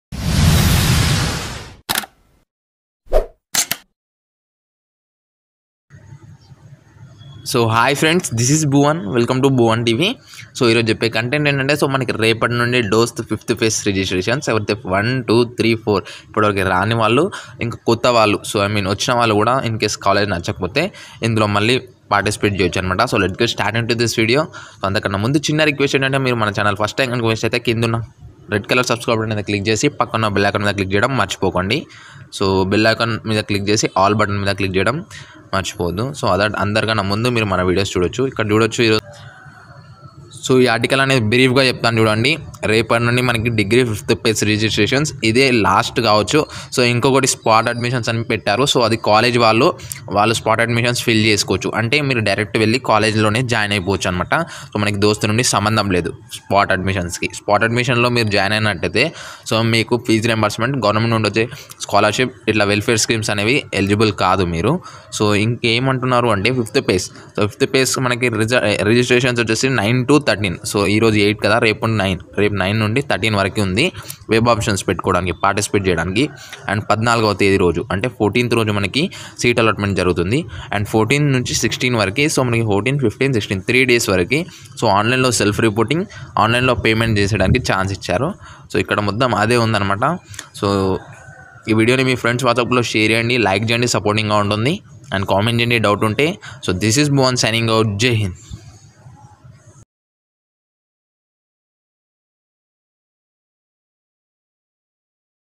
TS dost 5th phase notification released 2019|Ts dost spot admissions notification 2019|bhuwan tv
About video: in this video I'm saying about degree 5th phase notification and spot admissions details of telangana degree aspirants.